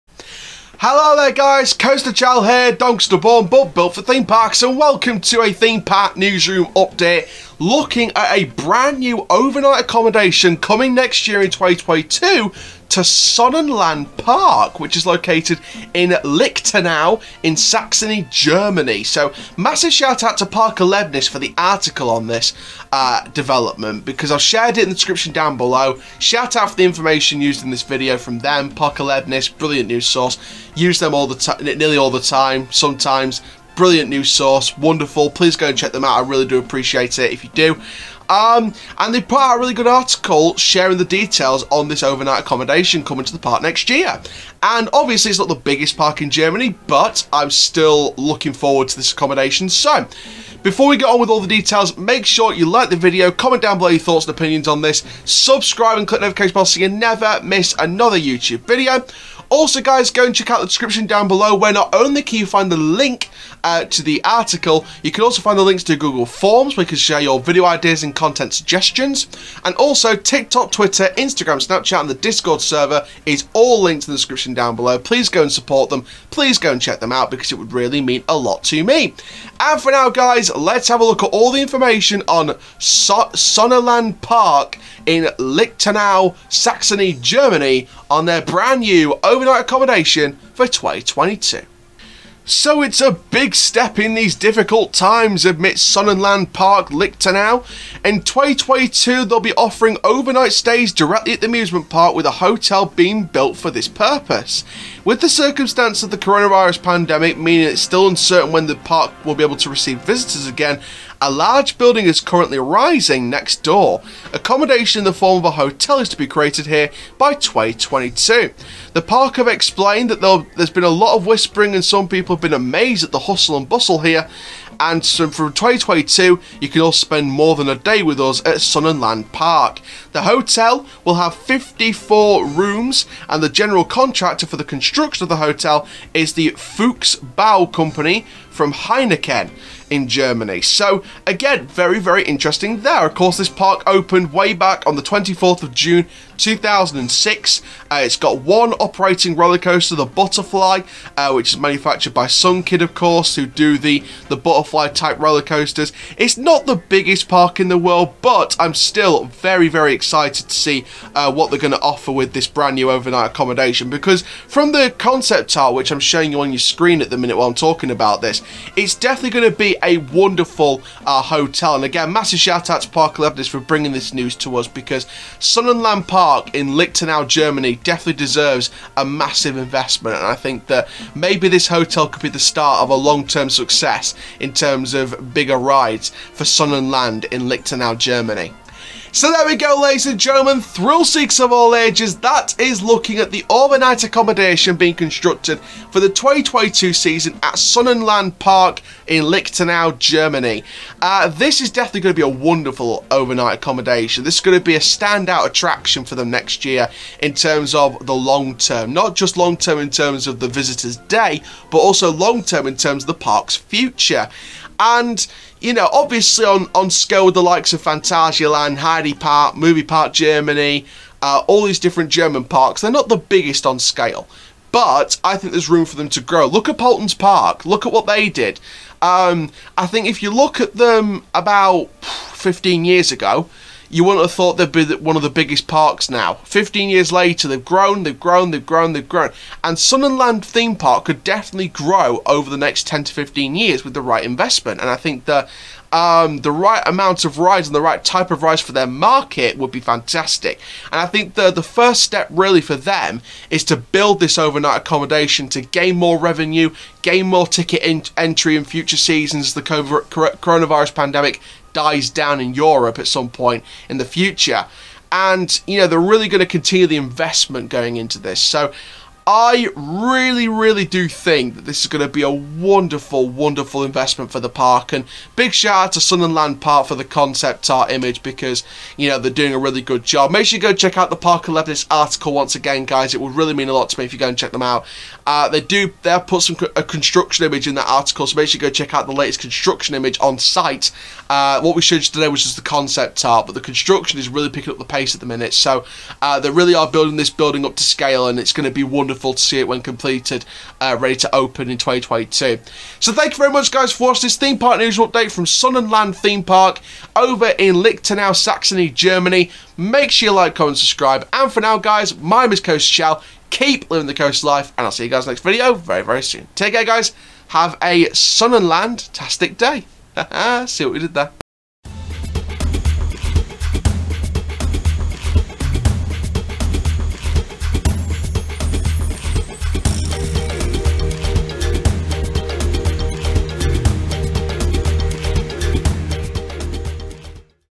Sonnenlandpark Lichtenau Has Revealed They Will Build A New Hotel For the 2022 Season
On this episode of Theme Park Newsroom, I'll be sharing news on a new hotel opening in 2022 at Sonnenlandpark, located in Lichtenau, Saxony, Germany. I'll also share my thoughts on this new onsite accommodation and what it could do to help the long term future of the theme park.

Intro: 0:00
Facts and Stats: 1:31
My Thoughts: 3:08
Outro: 5:17